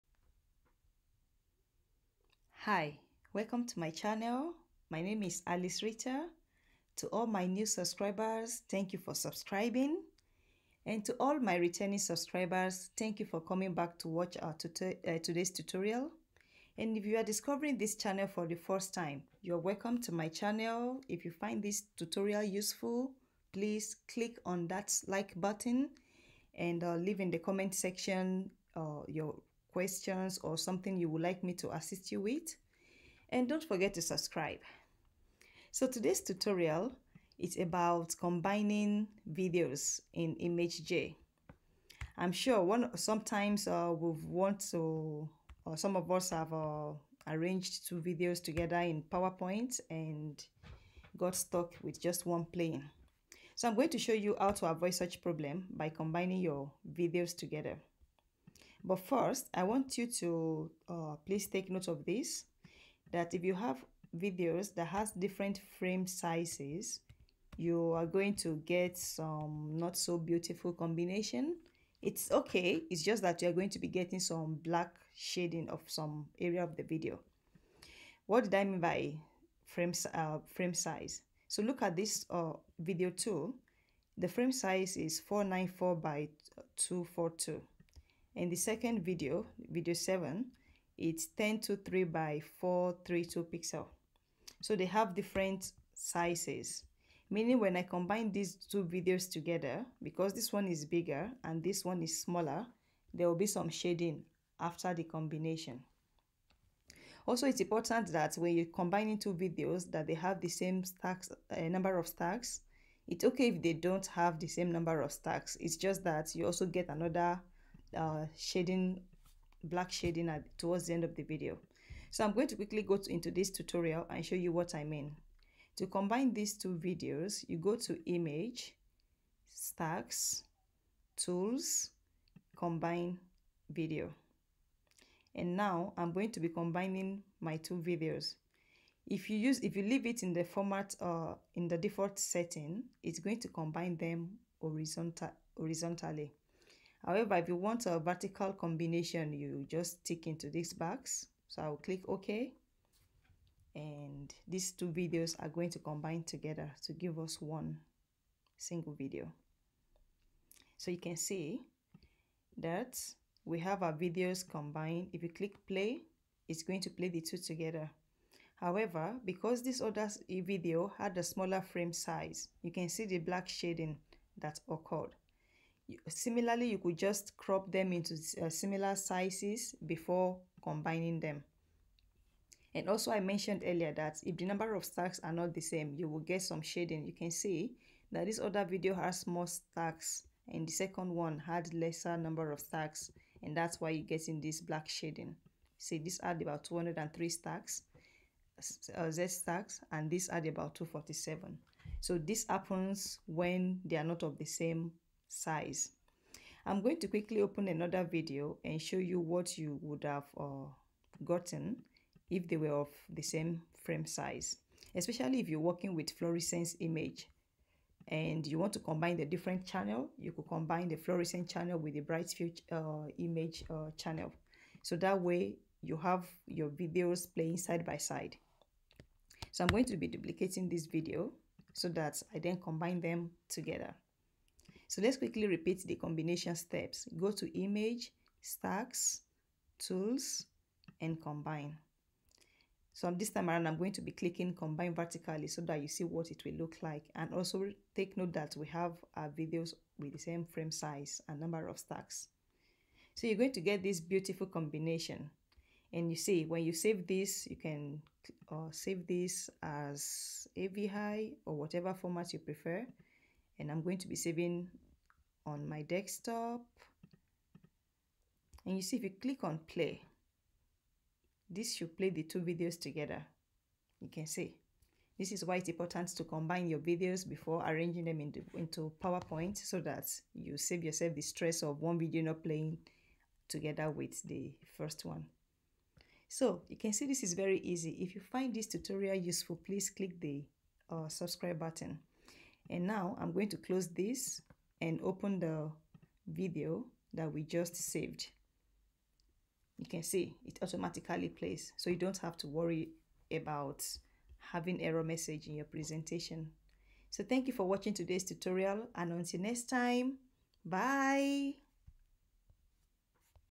How to combine videos in imageJ | side by side combination of fluorescence and brightfield video
This tutorial shows how to combine videos side by side in ImageJ. You can either vertically or horizontally combine the videos.